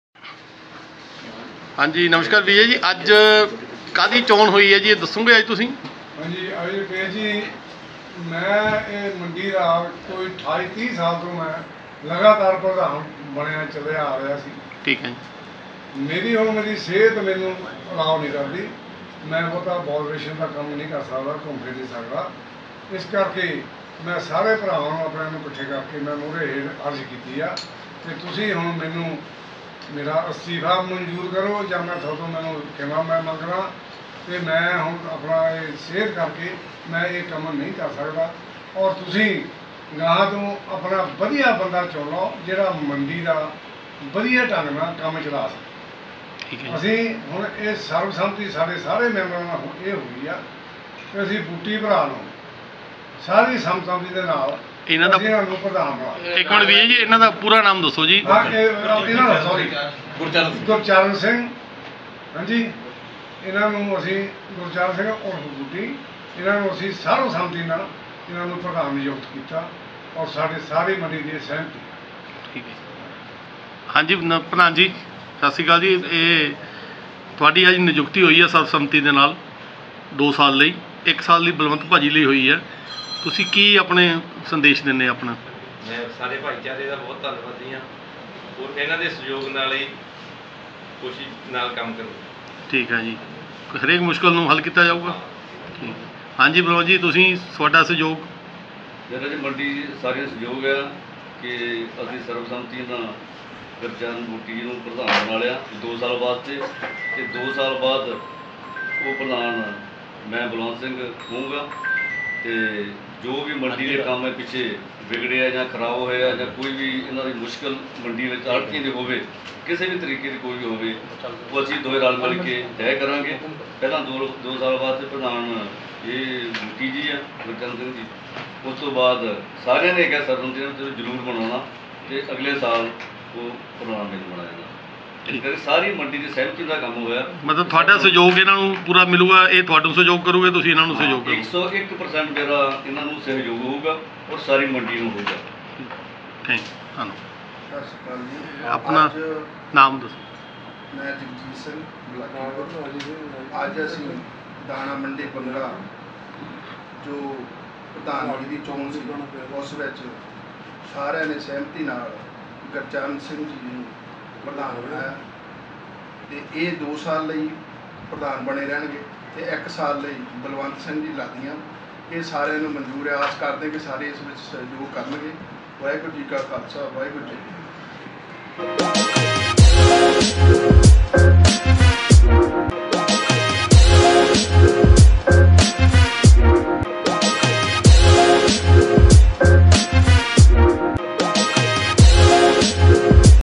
ਬੰਗਾ : ਸਰਬ ਸੰਮਤੀ ਨਾਲ ਬੰਗਾ ਤੋਂ ਆੜਤੀਆਂ ਐਸੋਸੀਏਸ਼ਨ ਦੇ ਕੀਤੇ ਗਏ ਪ੍ਰਧਾਨ ਨਿਯੁਕਤ !
ਬੰਗਾ : ਸ. ਗੁਰਚਰਨ ਸਿੰਘ 2 ਸਾਲ ਲਈ ਤੇ ਸ. ਬਲਵੰਤ ਸਿੰਘ ਲਾਦੀਆ 1 ਸਾਲ ਵਾਸਤੇ ਸਰਬ ਸੰਮਤੀ ਨਾਲ ਬੰਗਾ ਤੋਂ ਆੜਤੀਆਂ ਐਸੋਸੀਏਸ਼ਨ ਦੇ ਕੀਤੇ ਗਏ ਪ੍ਰਧਾਨ ਨਿਯੁਕਤ !!
ਬੰਗਾ ਤੋਂ ਅਰੋੜਾ ਦੀ ਰਿਪੋਰਟ !!